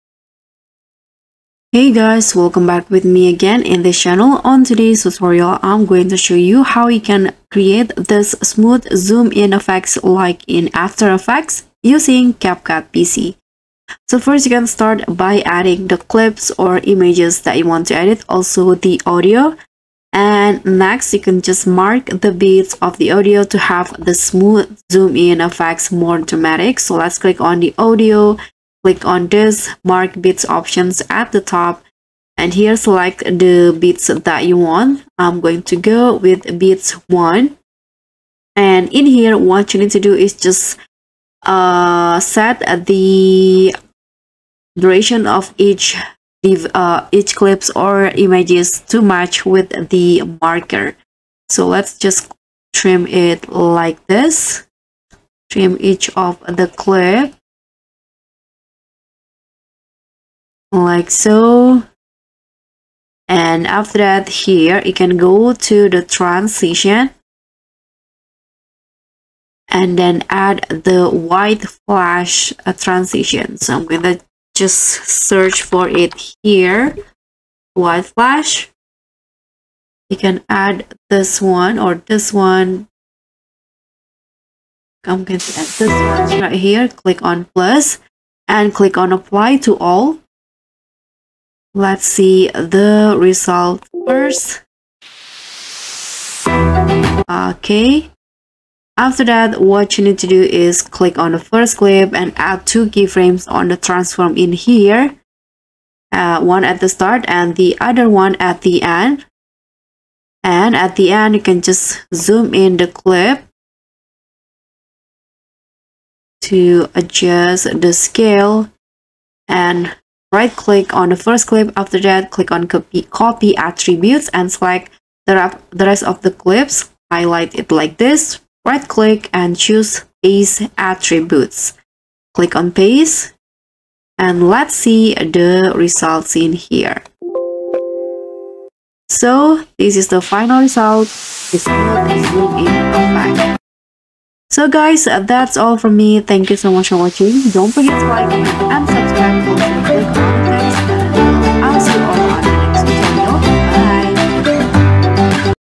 🔥 GUIDE: CapCut PC Tutorial: Creating Smooth Zoom In Effects Like After Effects | Step-by-Step Guide
Wanna learn how to have smooth zoom-in effects? This is how you can easily create a smooth zoom-in effect on CapCut PC